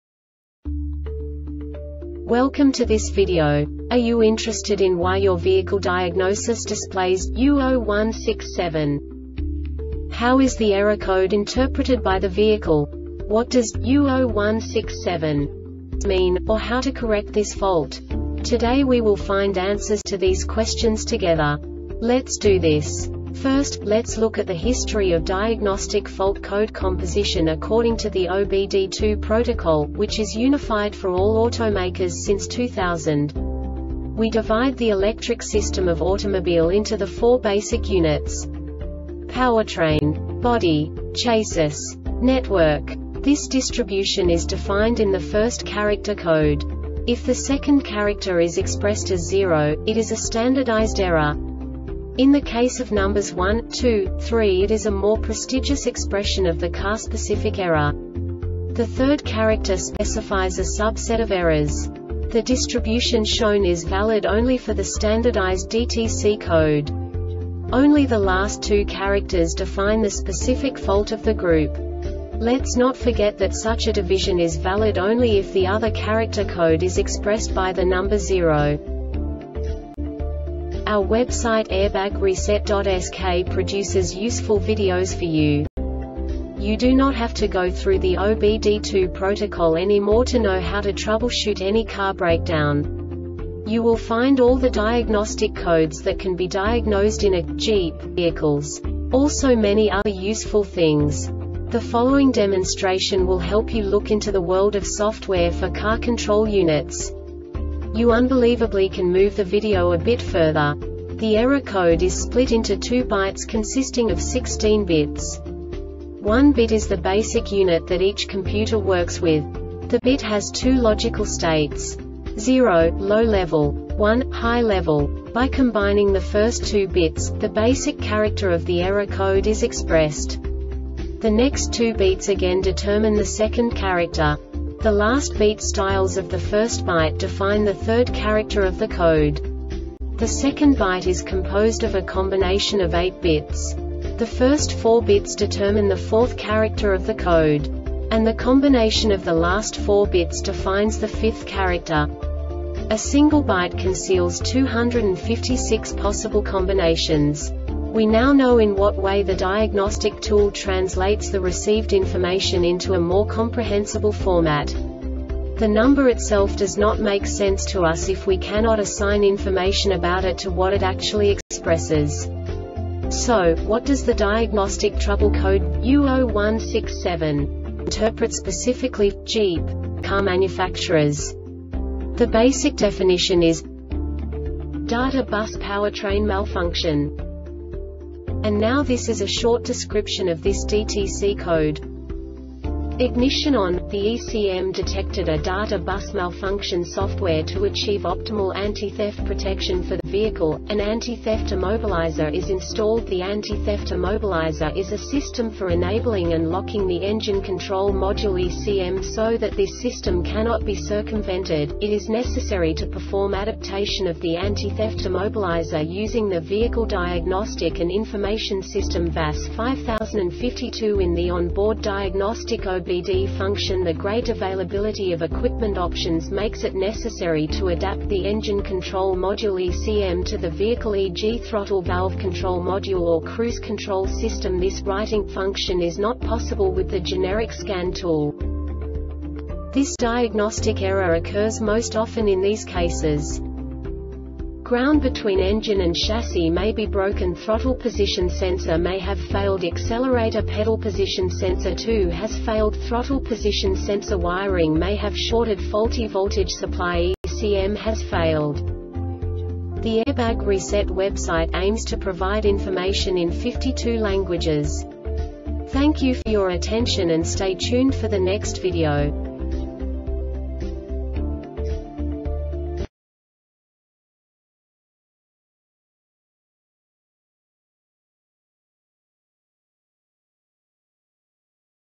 DTC Jeep U0167 Short Explanation
Contents:
0:21 Basic DTC analysis according to OBD2 protocol standard.
1:48 Insight into programming
2:58 A diagnostically understandable description of the Diagnostic Trouble Code
3:05 Basic error definition

Make:
Jeep
Code:
U0167
Definition:
Data Bus Powertrain Malfunction
Description:
Ignition on,the ECM detected a data bus malfunction (software).To achieve optimal anti-theft protection for the vehicle,an anti-theft immobilizer is installed.The anti-theft immobilizer is a system for enabling and locking the Engine Control Module (ECM).So that this system cannot be circumvented,it is necessary to perform adaptation of the anti-theft immobilizer using the Vehicle Diagnostic and Information System VAS 5052 in the On Board Diagnostic (OBD) function.The great availability of equipment options makes it necessary to adapt the Engine Control Module (ECM) to the vehicle (e. g.throttle valve control module or cruise control system).This “writing” function is not possible with the generic scan tool.
Cause:
 Ground between engine and chassis may be broken Throttle position sensor may have failed Accelerator Pedal Position Sensor 2 has failed Throttle position sensor wiring may have shorted Faulty voltage supply ECM has failed